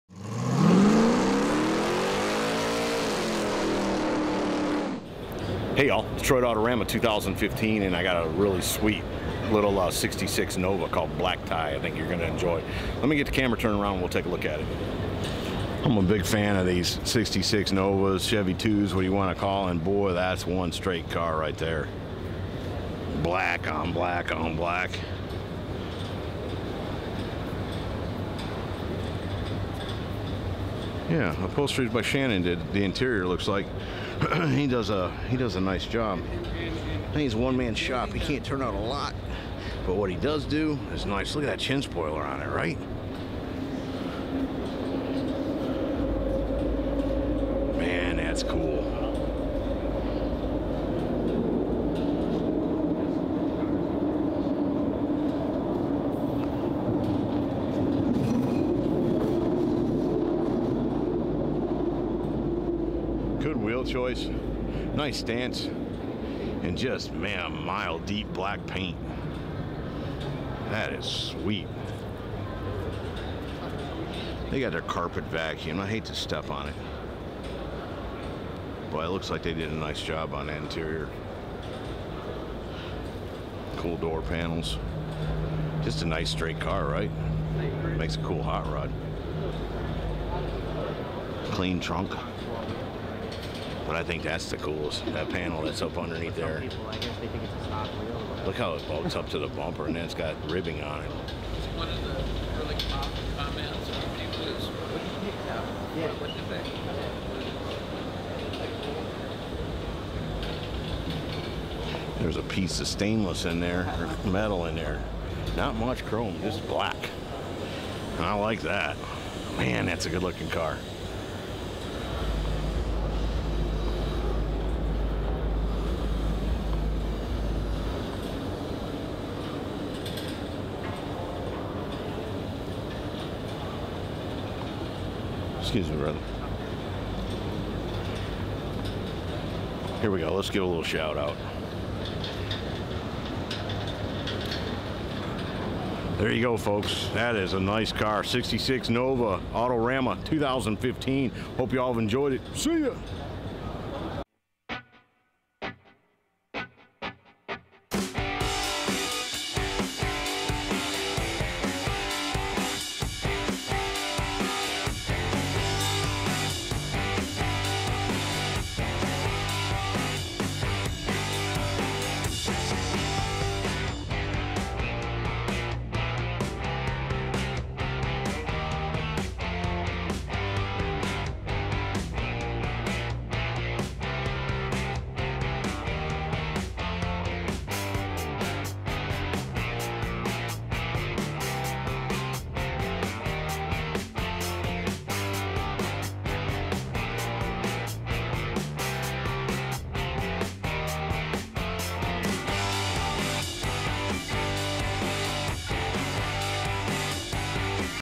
66 Nova "Black Tie" Detroit Autorama 2015
The 1966 Chevrolet Nova "Black Tie" from the 2015 Detroit Autorama...This is one cool Hot Rod. Check it out!!..Follow me for more new awesome videos weekly Mon-Fri!!
Check it out!!